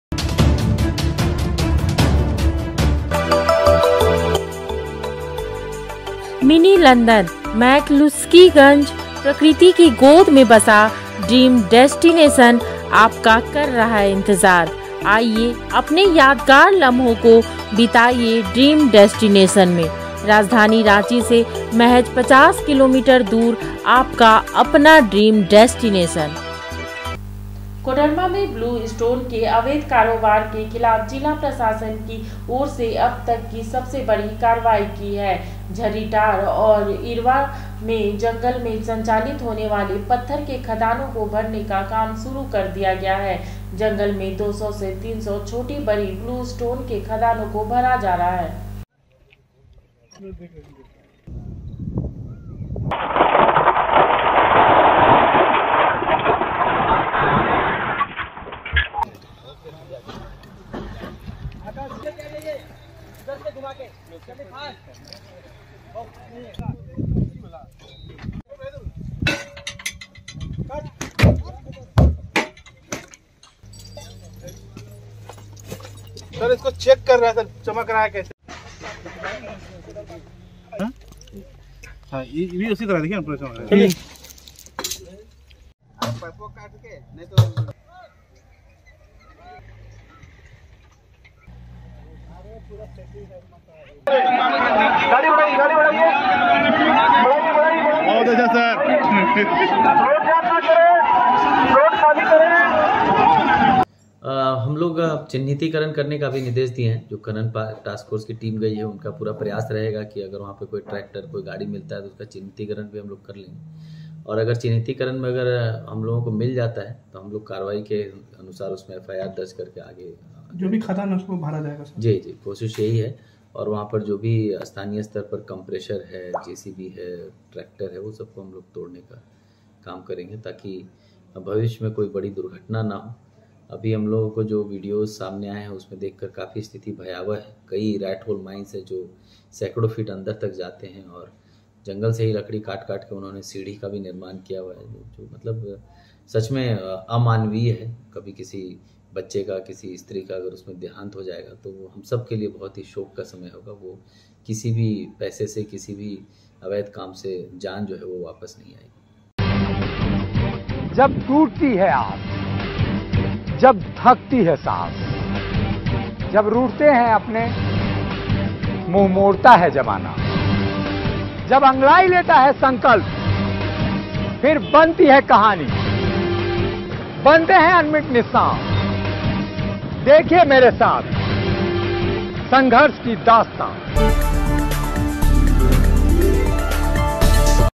कोडरमा में अवैध ब्लूस्टोन कारोबार के खिलाफ छापेमारी, खुदे पड़े खदानों को भरने का कार्य शुरू
notification.